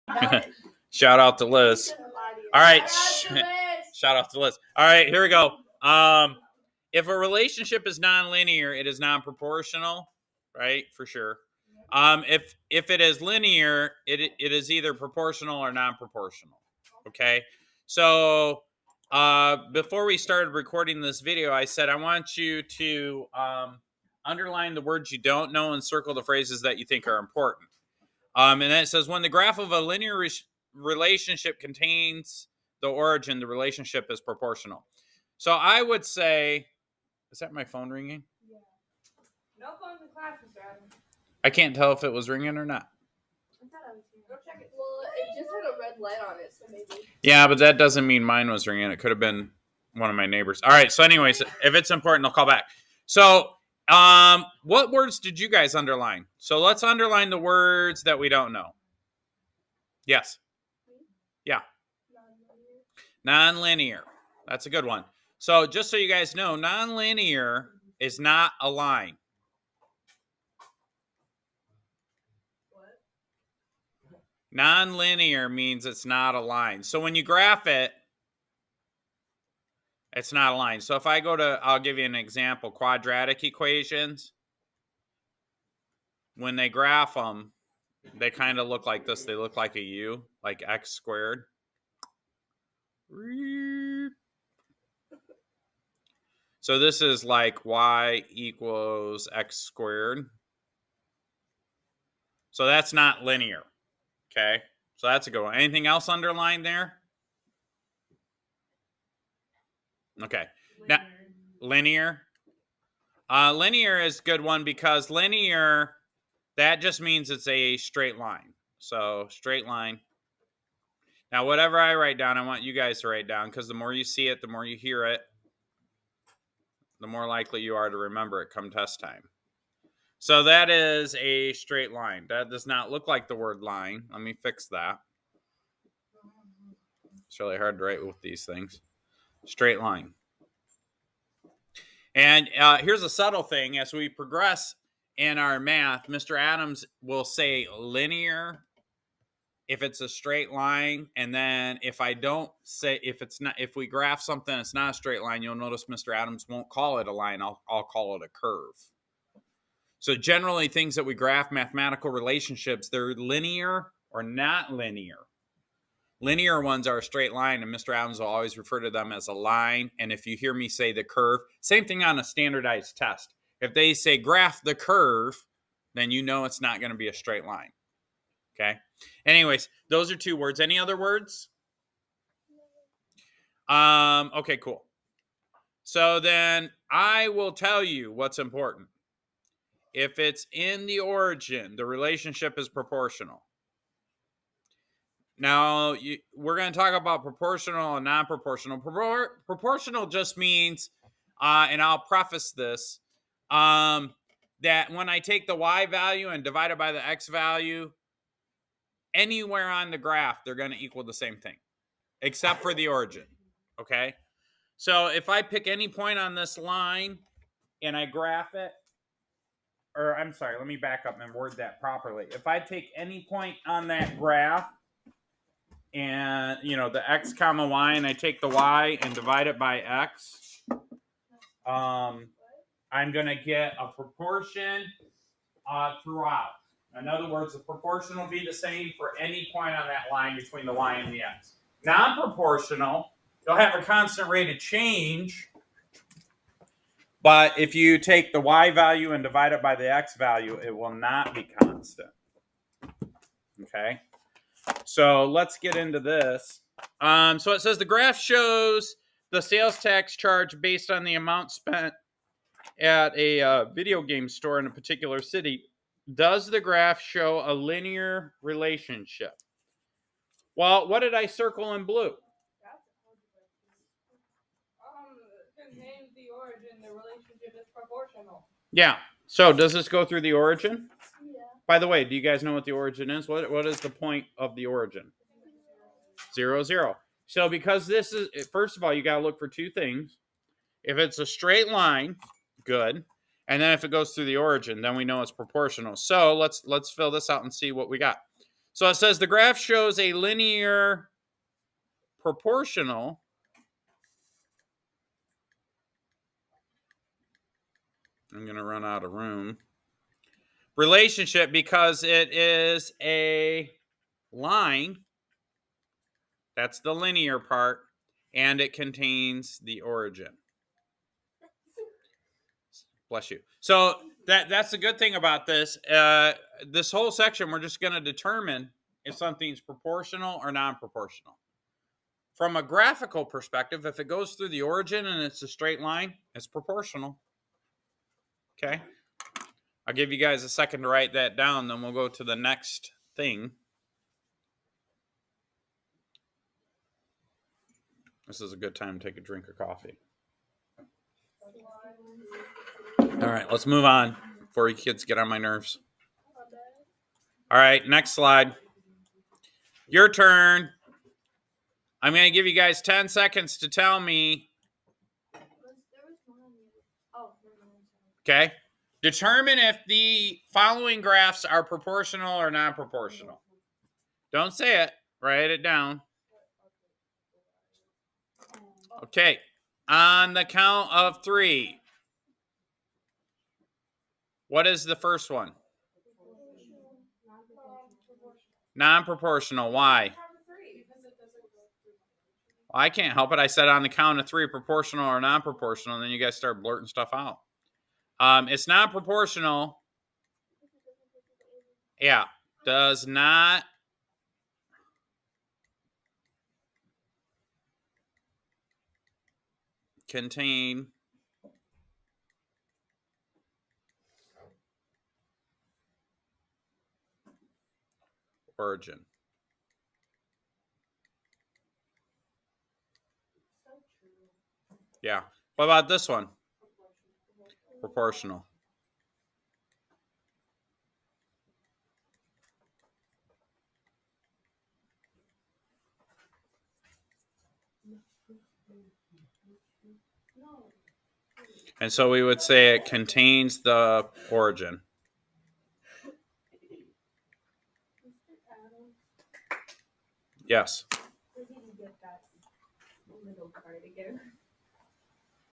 Go Math 8 4.4 Nonproportional and Proportional Example 1
In this tutorial, Tom Adams, a math teacher, teaches his student how to determine if a linear relationship is proportional or nonproportional.